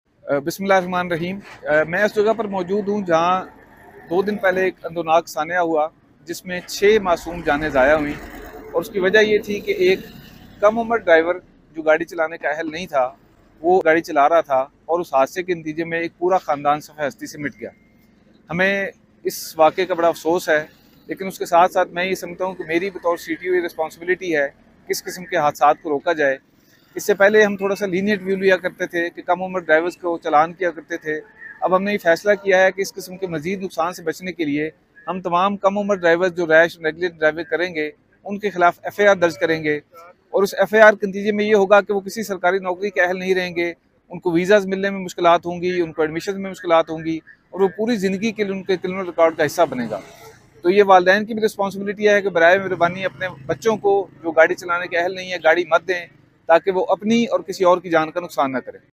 Accident ‼️ 12-11-2023 | DHA Lahore | 6 died | underage driver | CTO message | SSP | be careful
کم عمر ڈرائیور اگر پولیس کی گرفت میں آگئے تو صرف چالان نہیں ہوگا بلکہ FIR درج کی جائے گی جس سے ان کا ساری زندگی کا ریکارڈ خراب ہوجائے گا۔ والدین سے گزارش ہے کہ اپنے کم عمر بچوں کو سواری چلانے کی اجازت نا دیں۔ شکریہ
Accident 😢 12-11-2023 | DHA Lahore | 6 died | underage driver | CPO message | SSP | be careful